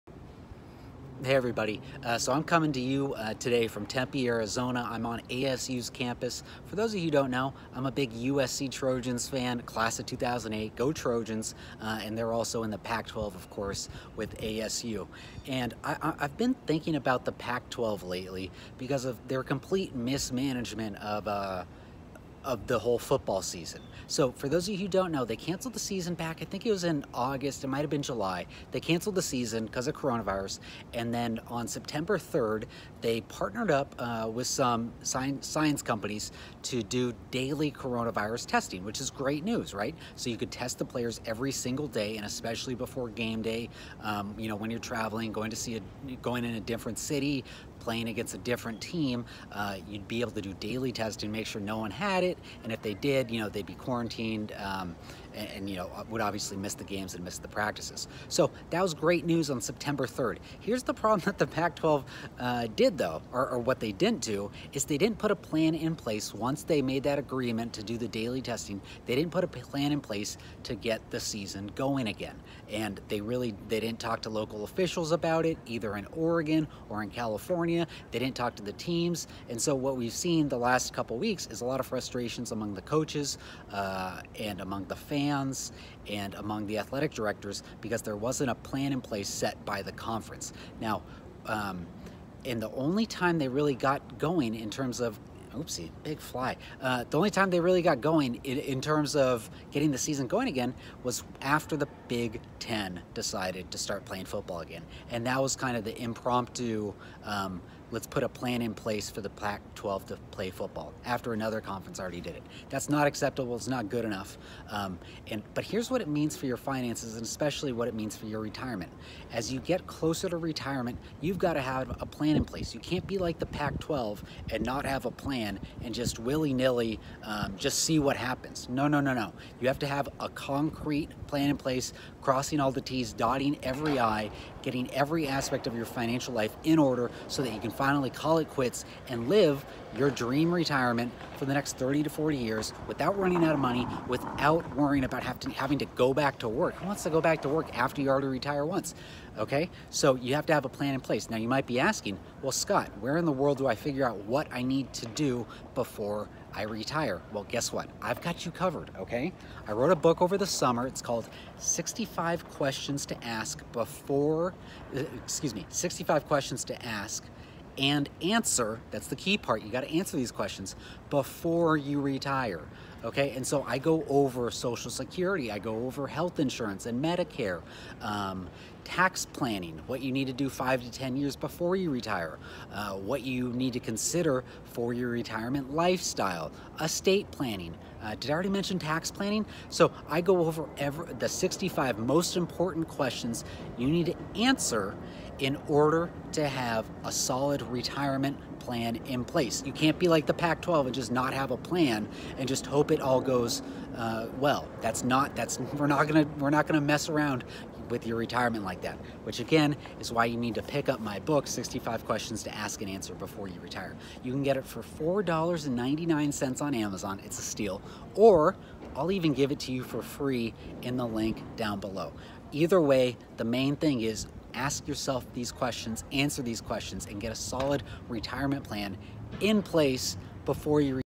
How The Pac 12’s Failure To Plan Can Help Your Retirement Plan
Sorry for the abrupt ending, you only missed one word!

2 – Grab a free copy of my books on Retirement Planning
3 – Check out my podcast, Retirement Clarity Radio (available on my or any podcasting platform).